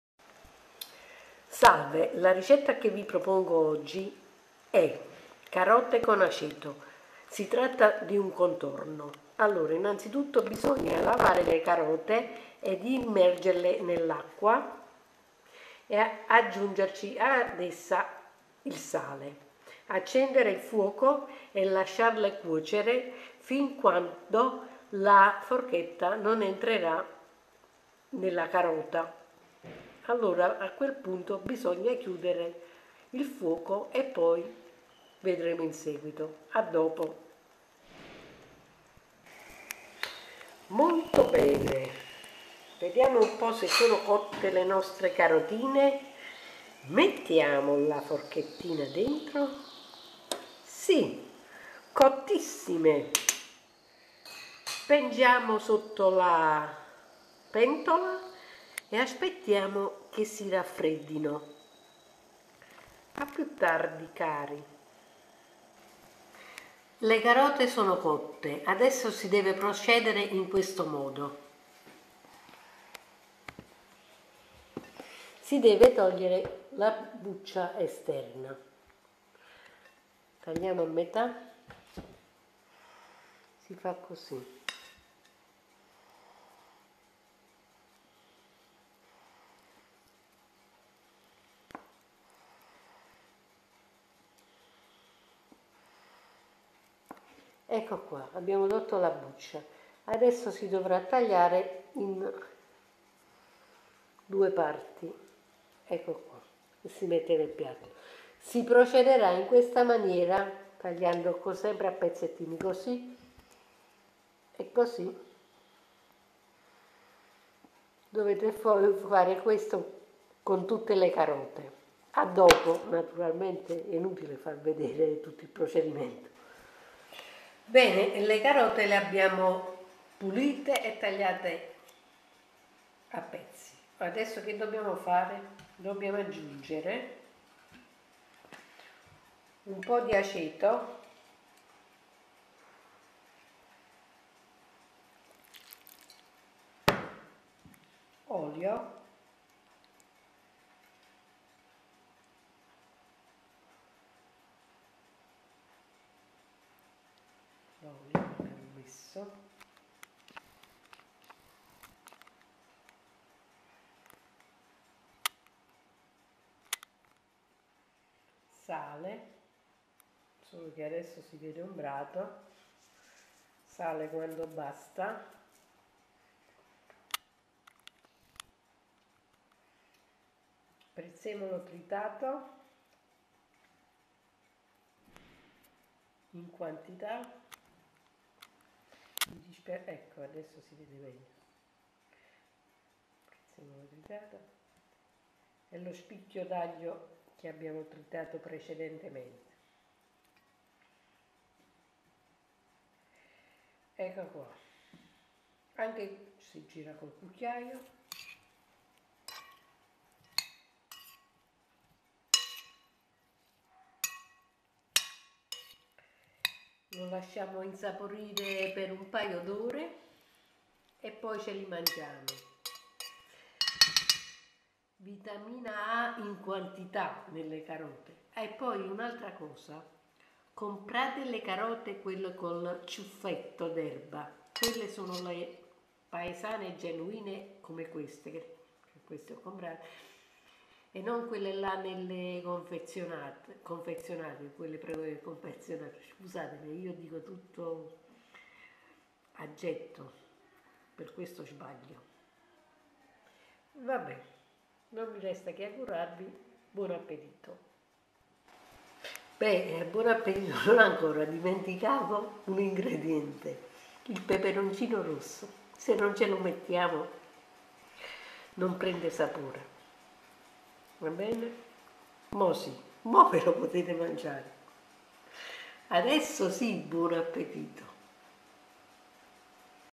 Carote all'aceto Contorno
Carote bollite e condite con olio, aceto e sale, ottime direi. Le carote: un ortaggio ricco di vitamina A percio' mangiamone a più non posso sono buone e saporite.